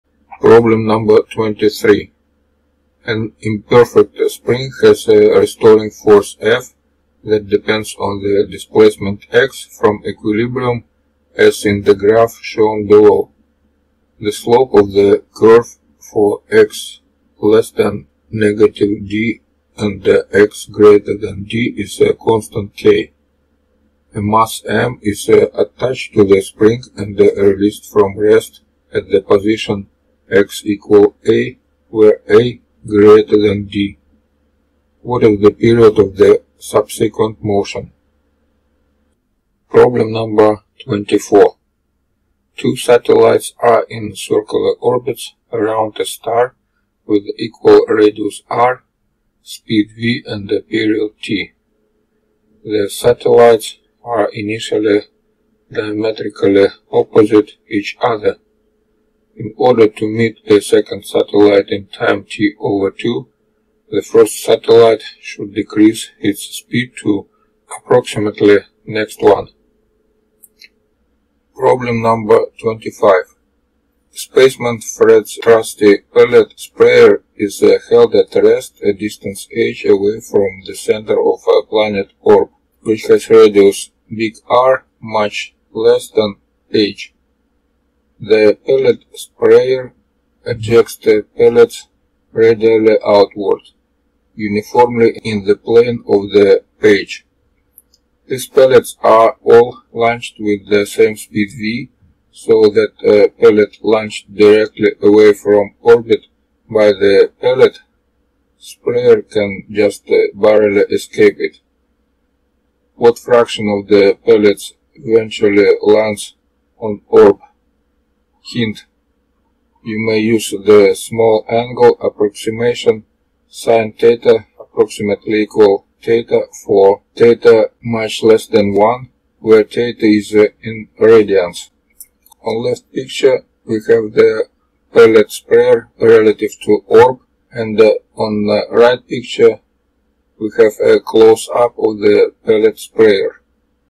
US F=ma Physics Olympiad 2021. First Round. Problems 23-25 of 25. Mechanics
In this video I presented problems 23-25 of 25  from 2021 year Olympiad F=ma

0:00 Problem 23
0:39 Problem 24
1:11 Problem 25